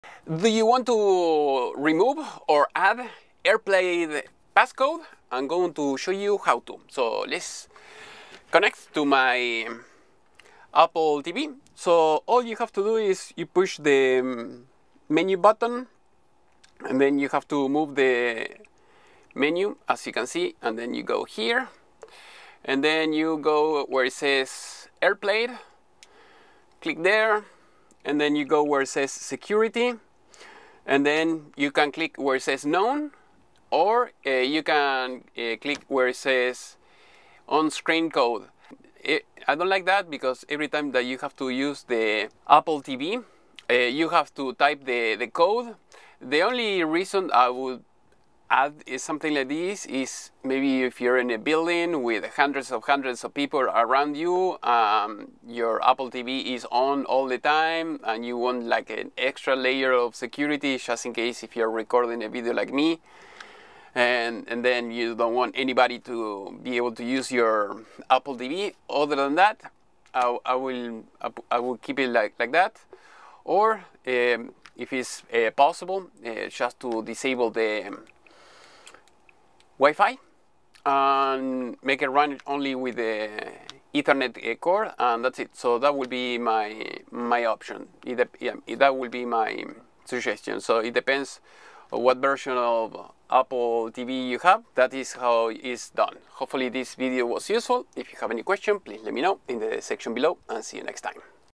How to disable or enable Airplay passcode Apple TV
Just go to Settings - AirPlay - Onscreen Code. There should be an option to disable it on or off. You can also take a look at manage AirPlay settings

Do you want to buy a course?
👋 SAY HI TO ME ON SOCIAL MEDIA!

QUESTIONS?
do you want to help me with some $$?